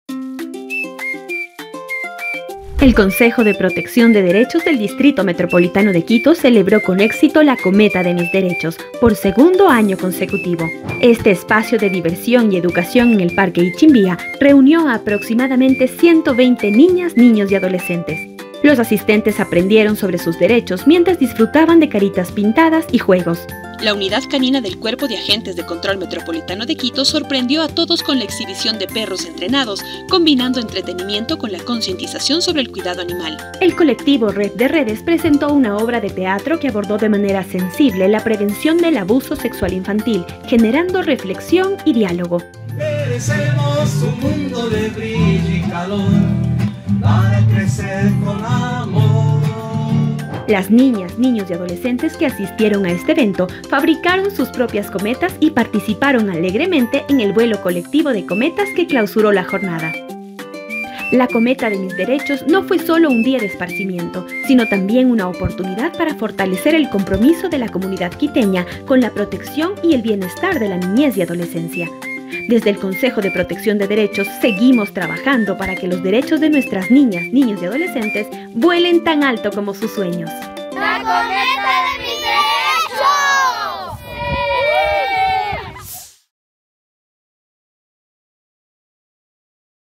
Resumen - La cometa de mis derechos
🪁 ElConsejoRenace | La Cometa de mis Derechos llevó sonrisas y reflexión a niñas, niños y adolescentes.
🎭 Aprendizaje lúdico sobre derechos.
🎨 Arte y creatividad.
🏃‍♂️ Diversión en comunidad.
Gracias a quienes hicieron posible este evento. Juntos por un QuitoConDerechos 🌟